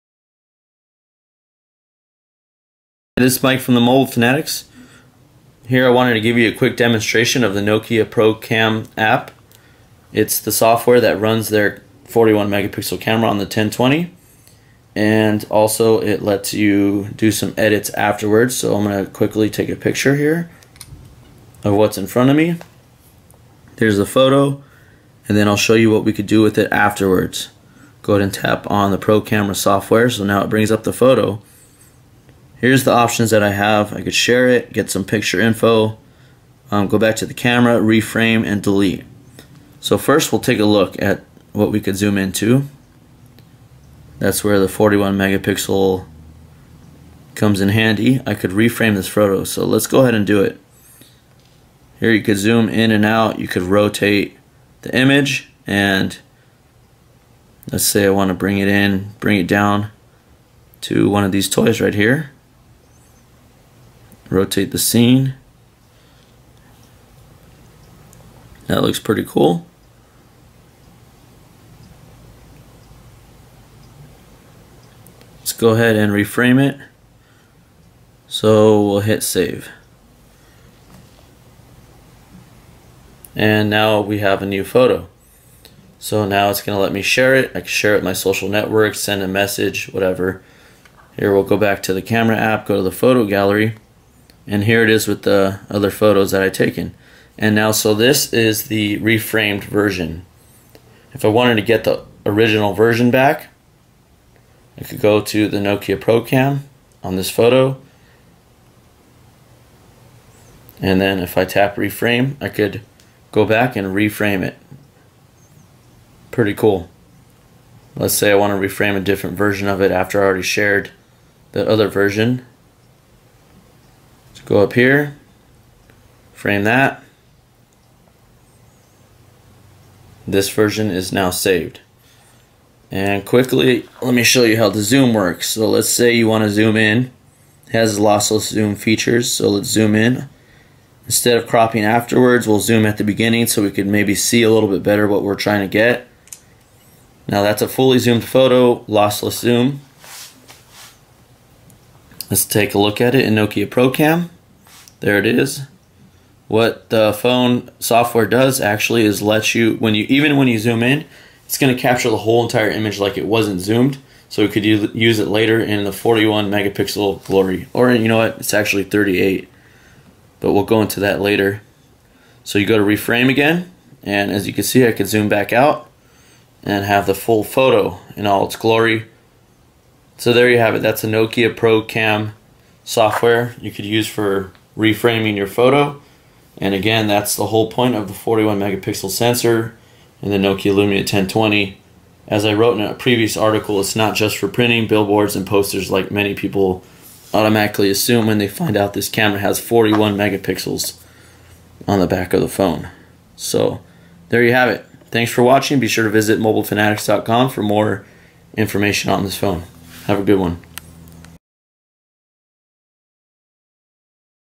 Nokia Lumia 1020 demo: Reframe 41 megapixel photos with Pro Cam
Here I give a demo review of the Nokia Pro Cam software, specifically the reframing and cropping features. It's the whole point of the 41 megapixel sensor.